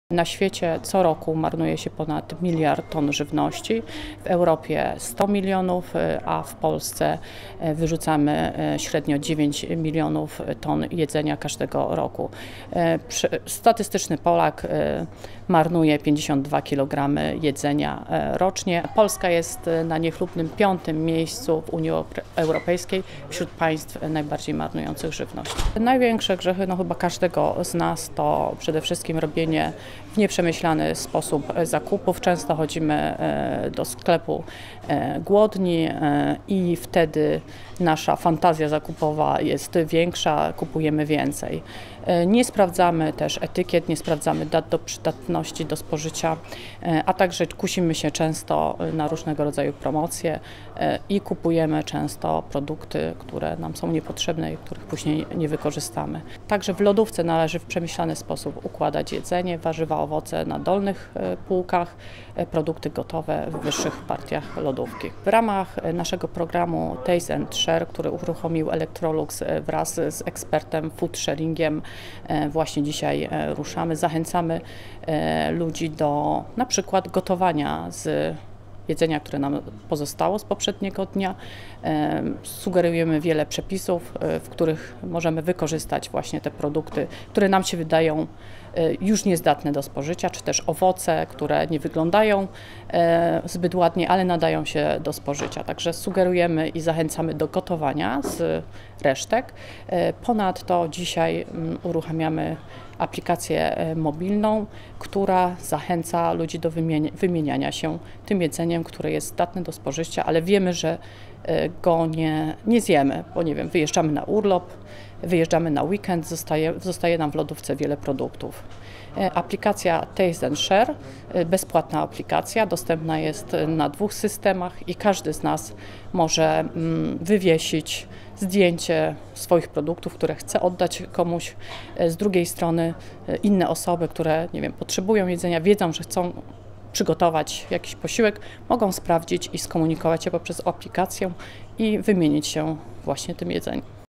6 trików pomoże ograniczyć wyrzucanie jedzenia
Wypowiedź: Dominika Kosman, rzecznik prasowy Electrolux.

W rok w naszym kraju marnują się aż 52 kg żywności na osobę. Polska jest w czołówce krajów europejskich, w których wyrzuca się najwięcej jedzenia. Co roku marnujemy około 9 mln ton żywności, z czego 2 mln ton wyrzucane jest przez gospodarstwa domowe – wynika z danych Banków Żywności. Jak unikać marnowania jedzenia?